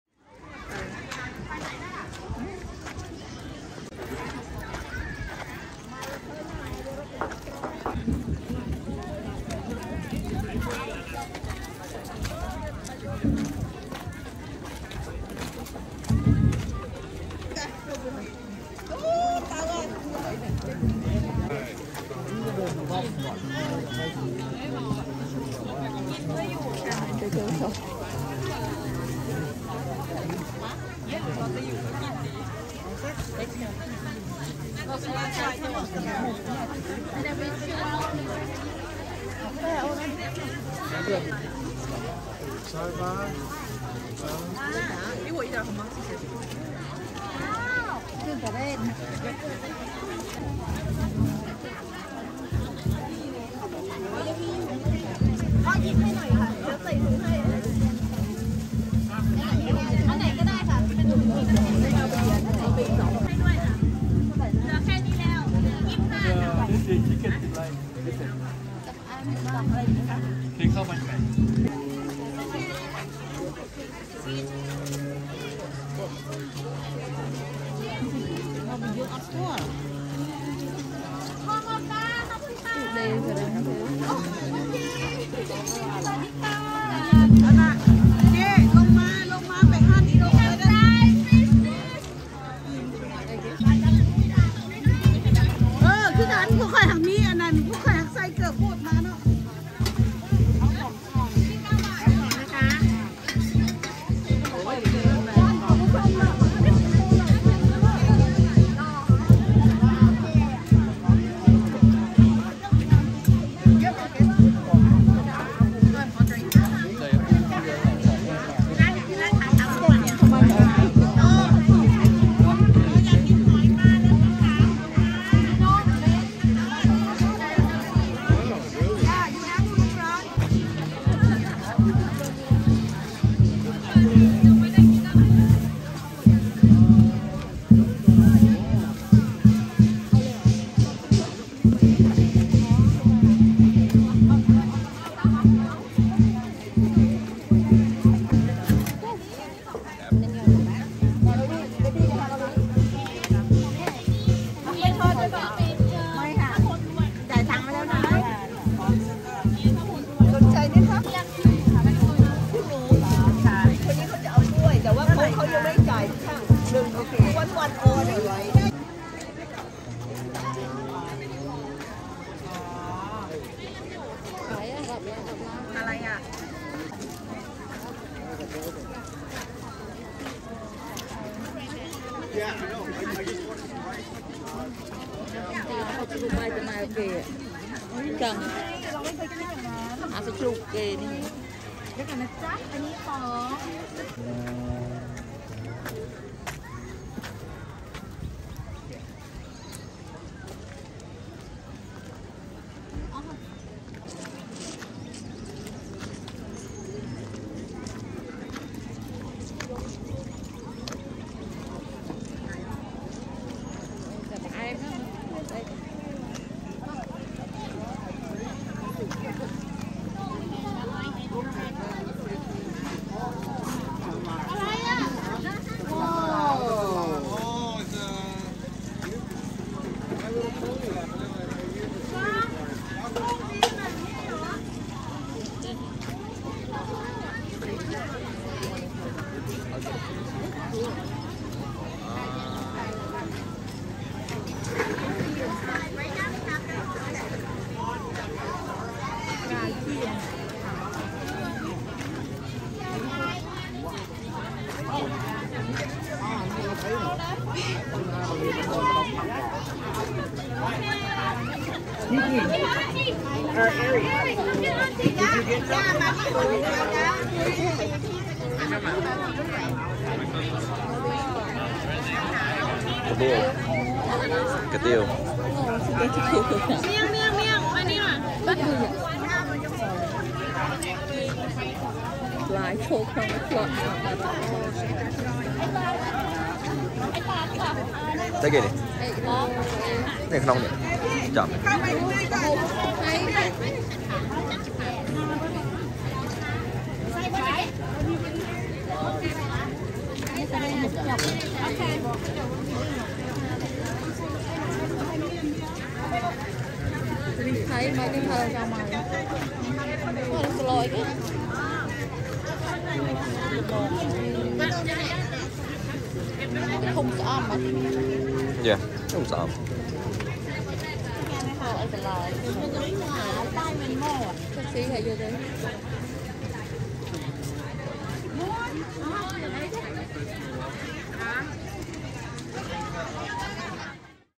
ចូលឆ្នាំនៅវត្តថៃ ២០២៣ Songkran Festival at Wat Nawamintararachutis on April 30, 2023
Songkran Festival on April 30, 2023
The temple is called Wat Nawamintararachutis. It is located in Raynham, MA